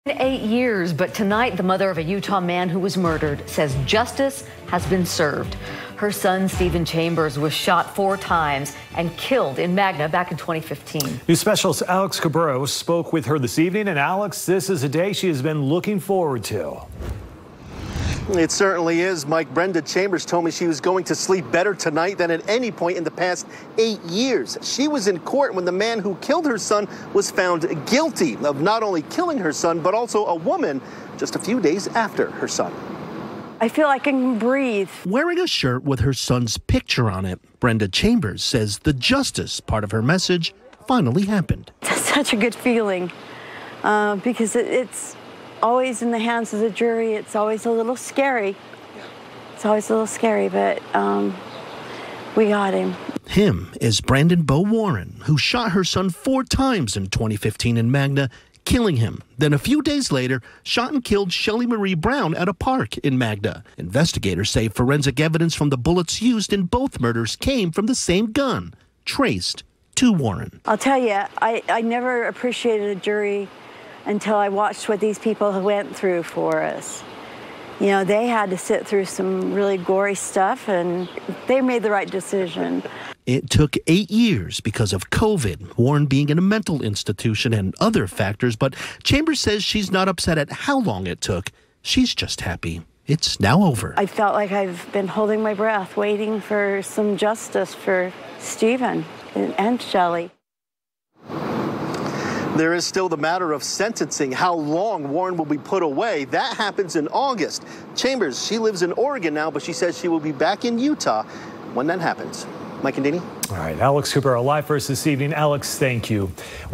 ‘It’s personal’: Jury finds Magna man guilty of 2 murders
Brenda Chambers wore a shirt saying “Justice for Stevan” on it, and she feels justice was granted Friday for her son, Stevan Chambers, when a 3rd District Court jury found Brandon Beau Warren, 31, guilty on two counts of murder, both first-degree felonies.